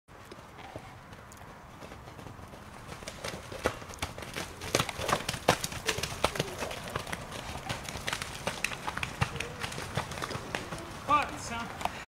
Maaike fan é Welle Ster (Rik x Teunis)
Maaike fan de Welle en haar dochter (6 weken te vroeg) Ietsje van de Noeste Hoeve